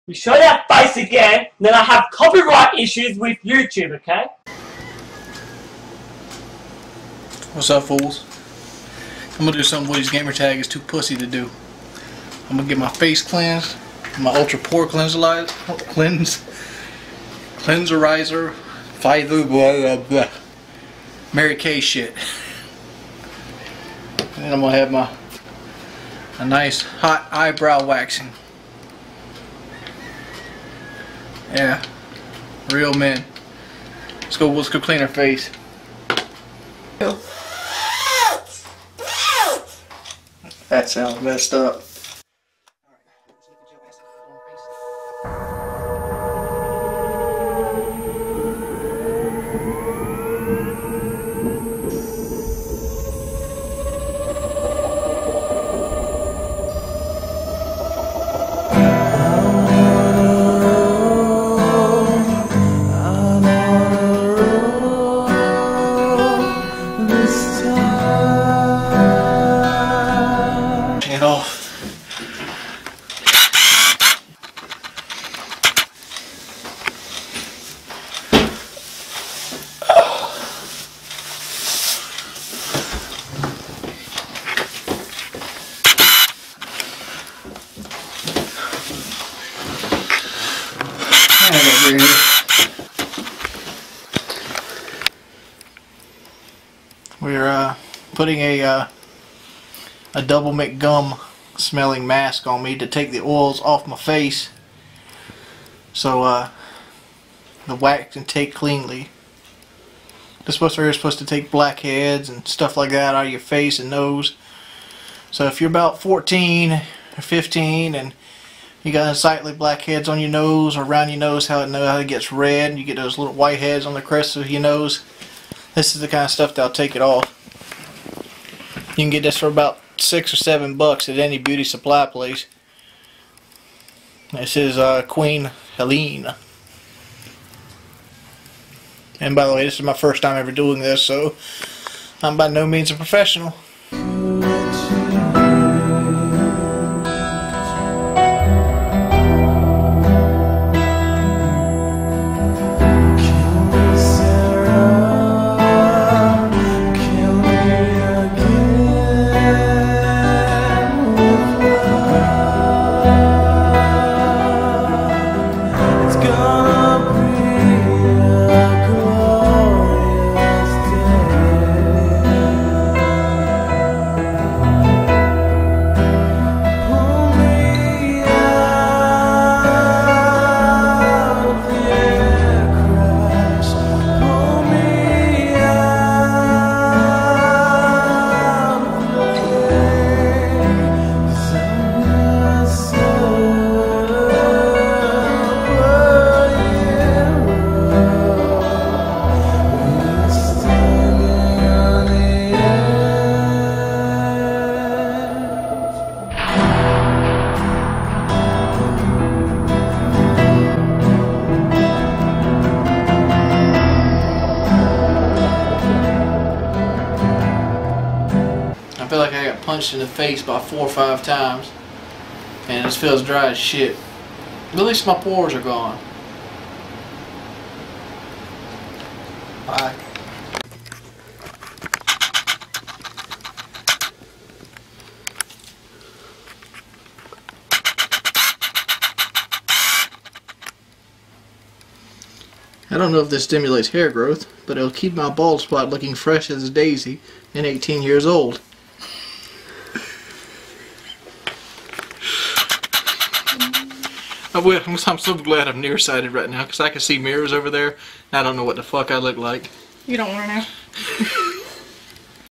WingsOfRedemption aka Jordie Jordan becomes a Beauty YouTuber
From the Archives, more coming soon?